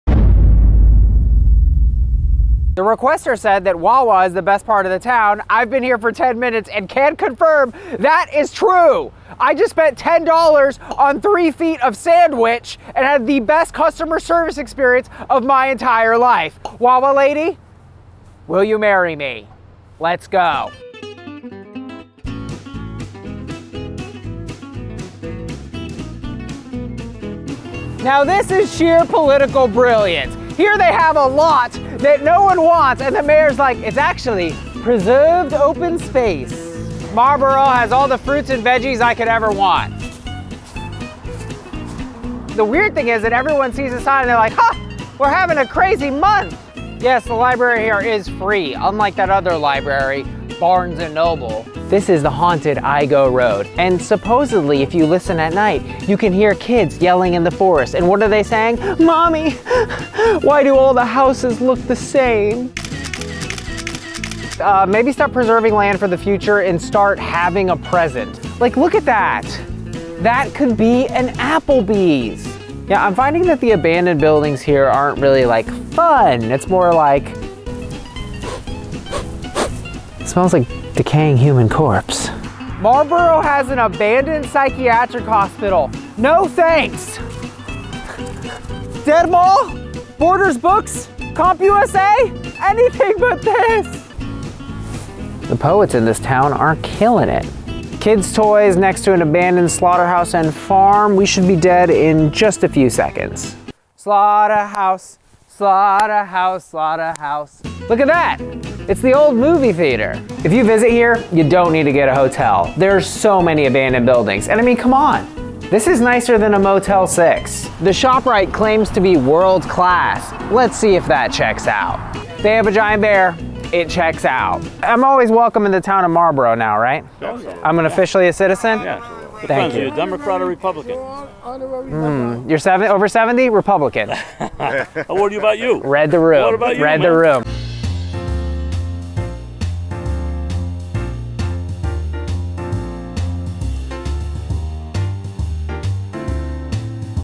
Where Wawa Is The Best Thing For Miles (Marlboro, NJ)
Marlboro, New Jersey's abandoned buildings will leave you terrified: the slaughterhouse, psychiatric hospital, and creepy Igoe road, let's grab Wawa and GTFO.

***Episode Notes***
Request number 2 was sick as hell. Marlboro proved to be the CG of New Jersey though way freaking creepier. For my money, Wawa has the best convenience store food I've ever encountered, though beverage selection is superior at Arizona fave, Circle K (QT is solid too though). You da best!